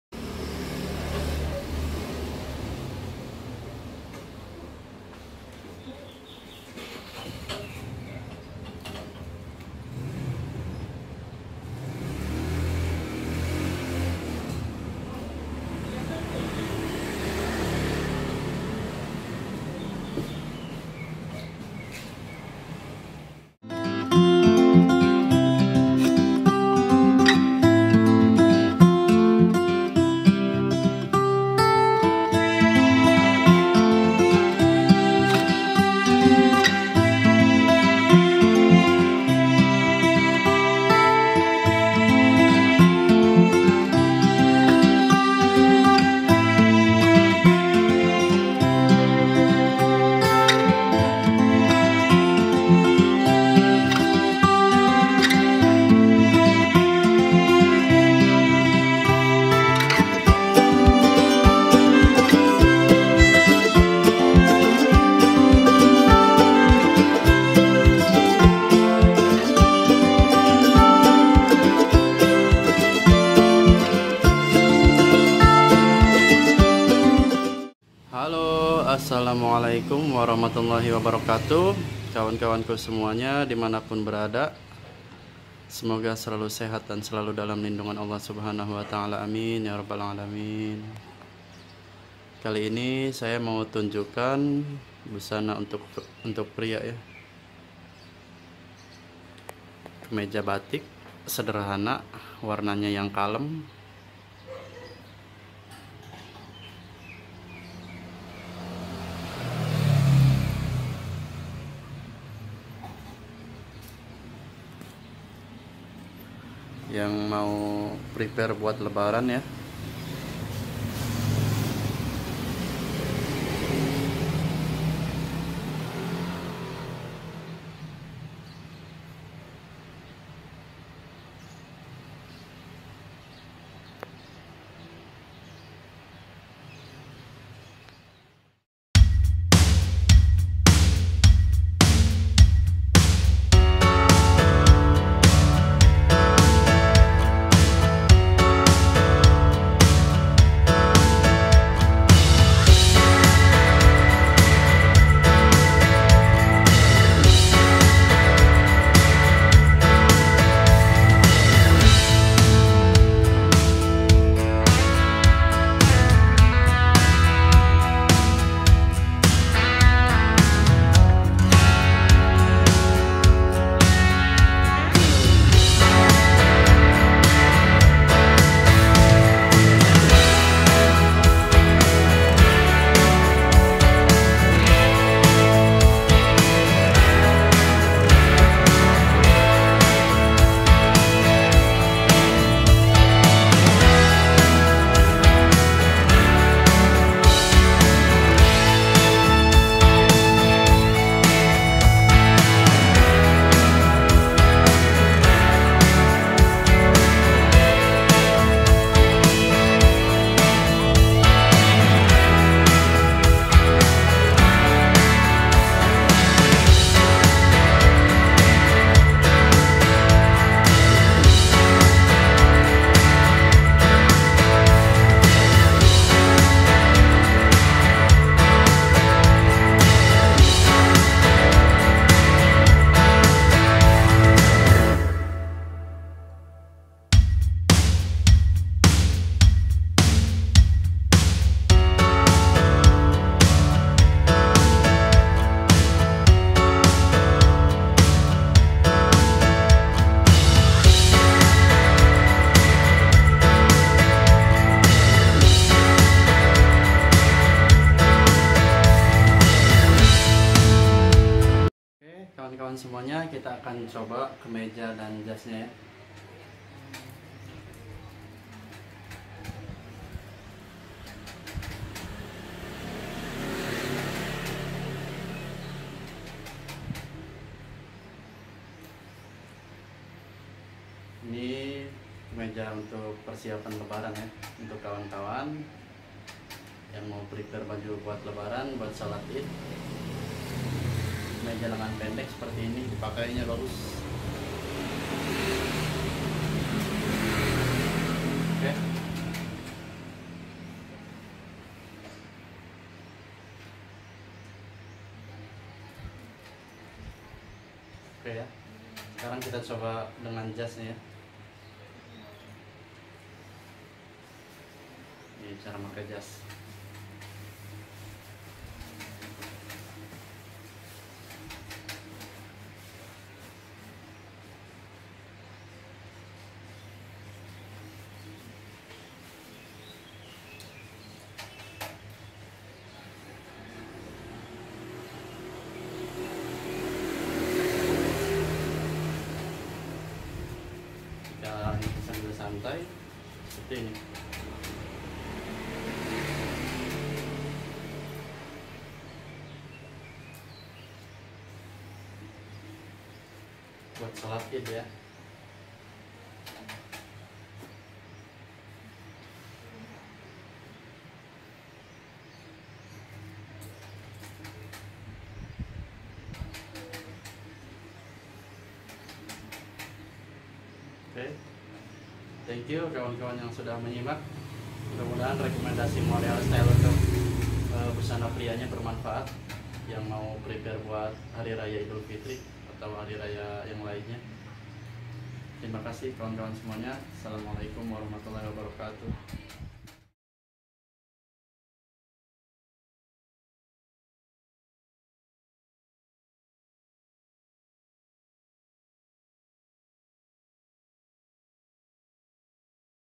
Rekomendasi busana pria.kemeja batik dan jas sederhana
assalaamualaikum warahmatullahi wabarakatuh kawan-kawan semuanya kali ini saya review dan rekomendasikan untuk model busana pria khususnya kemeja batik yang simpel dan modis dan jas modis untuk segala acara baik santai ataupun resmi untuk warna bisa disesuaikan dengan selera semoga rekomendasi ini bermanfaat untuk kawan-kawan semuanya khususnya yang sedang mempersiapkan busananya menyambut hari raya Idul Fitri di tahun ini semoga kita semua selalu sehat dan selalu dalam lindungan Allah dan bisa bertemu dengan Ramadhan dan Idul Fitri tahun ini sukses selalu untuk kawan-kawan semuanya terima kasih
untuk info kritik dan saran di nomor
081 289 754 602

terima kasih
yudi setiawan